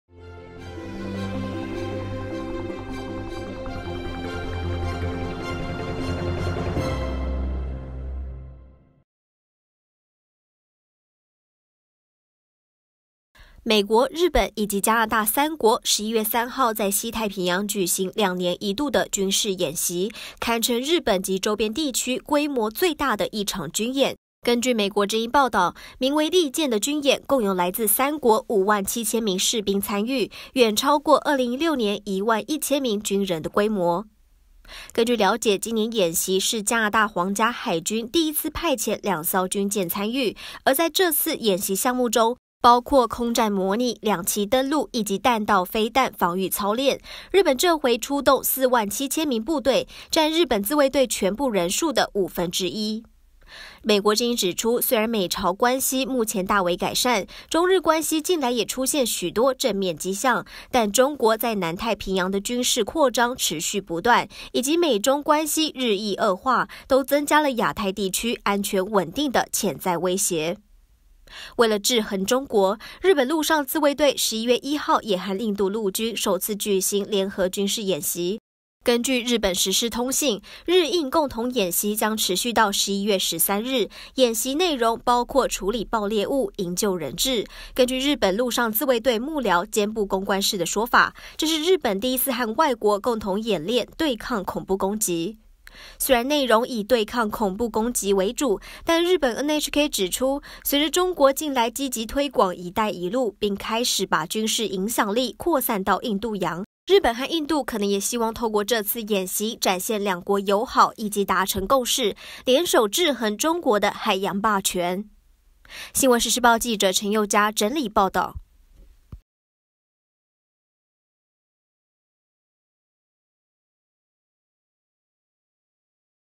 新闻时时报｜制衡中国海洋霸权，美日加联合军演登场（20181104）
美国、日本及加拿大三国，11月3号在西太平洋举行两年一度的军事演习，堪称日本及周边地区规模最大的一场军演。根据美国之音报导，名为利剑的军演，共有来自三国5万7千名士兵参与，远超过2016年1万1千名军人的规模。

报导指出，虽然美朝关系目前大为改善，中日关系近来也出现许多正面迹象，但中国在南太平洋的军事扩张持续不断，以及美中关系日益恶化，都增加了亚太地区安全稳定的潜在威胁。

明镜火拍记者：陈又嘉